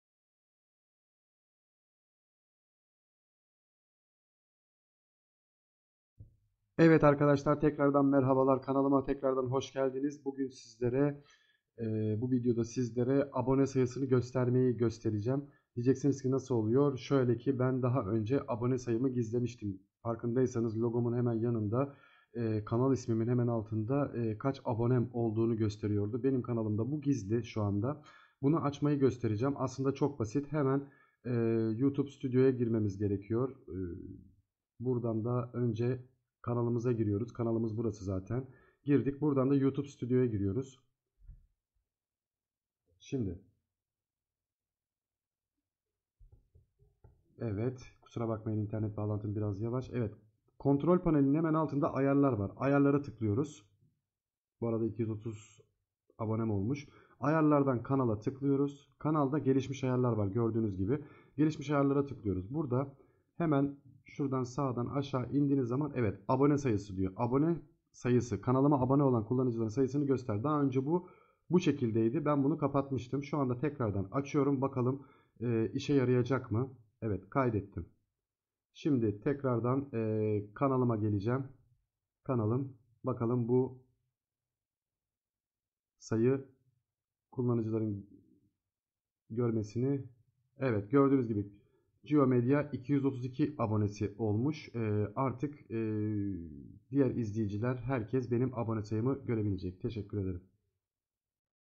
Abone Sayısını Gösterme
Yotube Kanalınızın aboneleri gözükmüyorsa bunu yapmanız yeterli olacaktır. Bu sayede abone sayısını gösterme ile ilgili sorununuz kalmayacaktır.